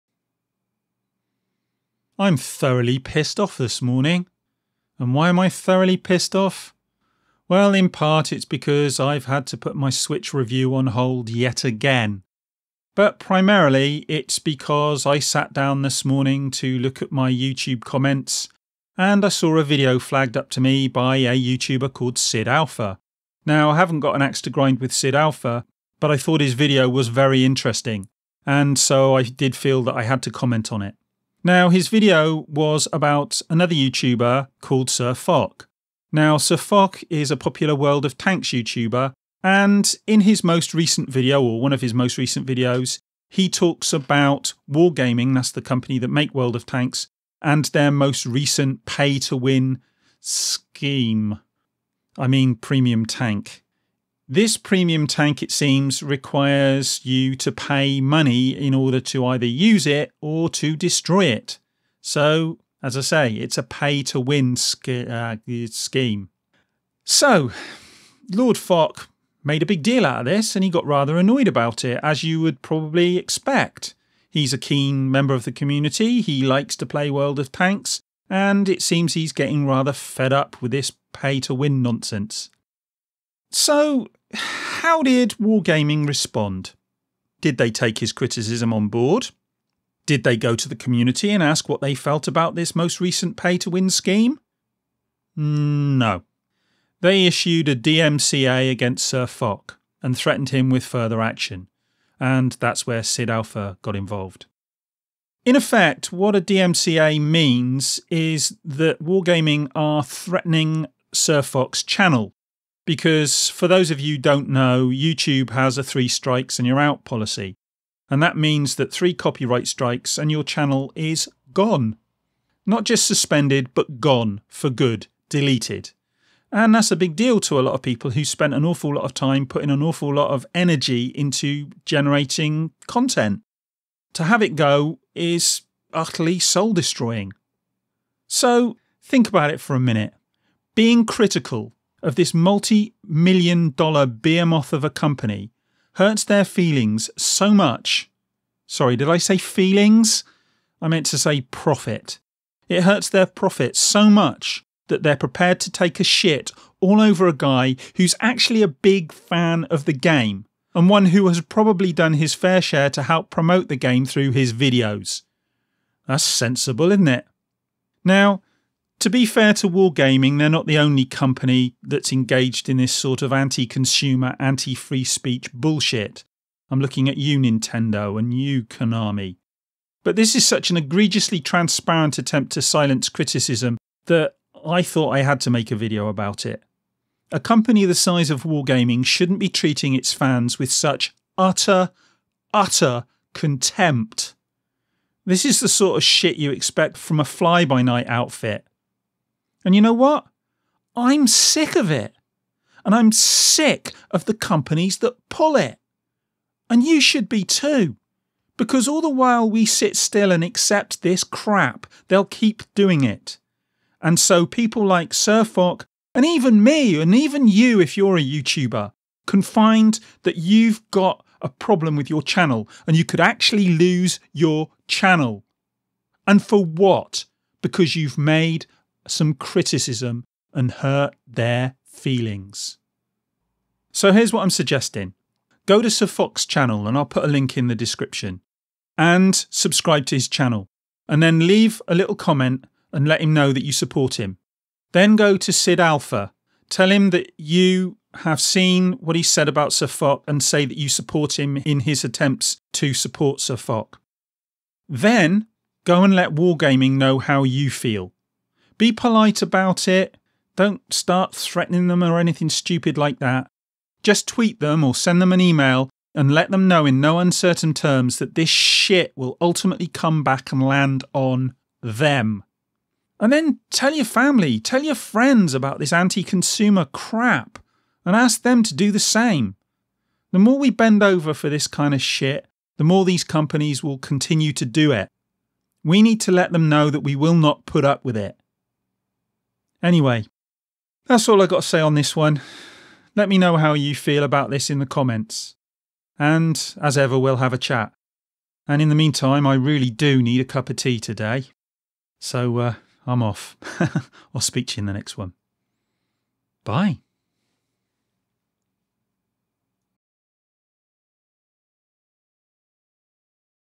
World of Tanks Creator Wargaming Can Foch Off
World of Tanks Developer Wargaming, threatened a You Tuber with a DMCA strike unless he took down his video criticising their latest, money grabbing scheme (sorry, I mean tank).

I don't know about you, but I don't think that's acceptable.